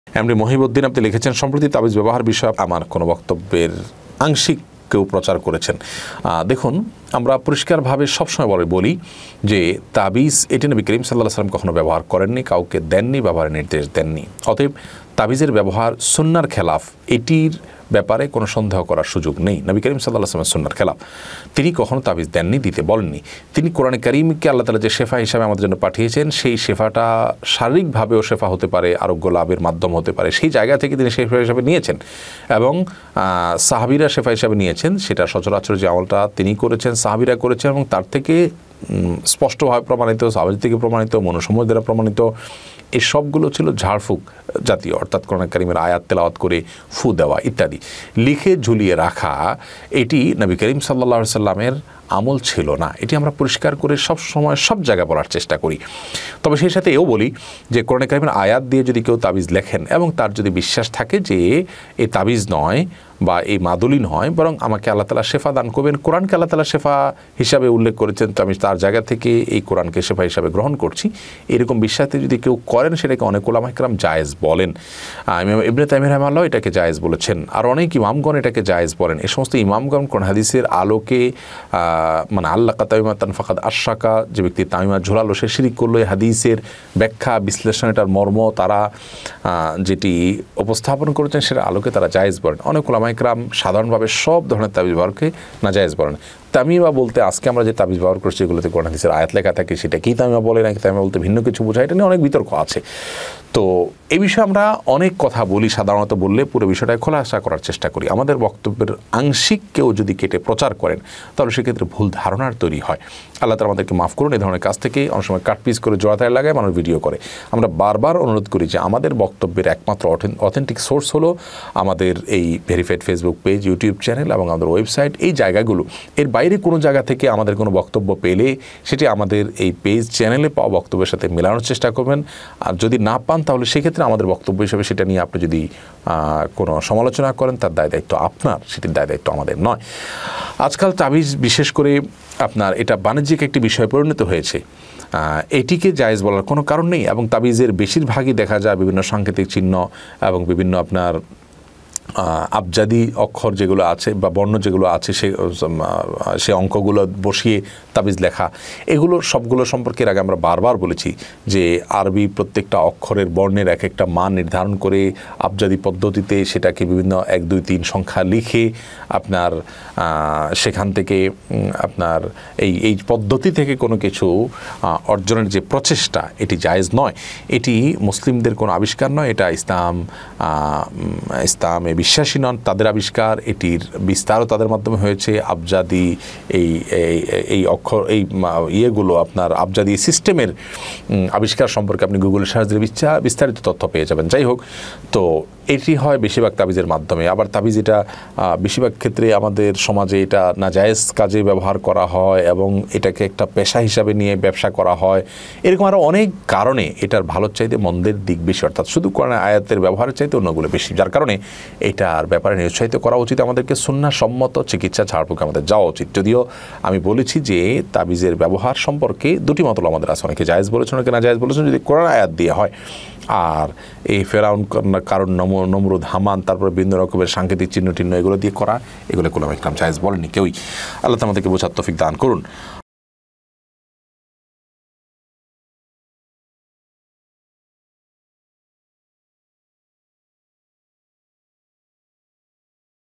তাবিজ বিতর্কের যে সুন্দর সমাধান দিলেন শায়খ আহমাদুল্লাহ
তাবিজ বিতর্কের যে সুন্দর সমাধান দিলেন শায়খ আহমাদুল্লাহ! শরয়ী সমাধান II Sheikh ahmadullah live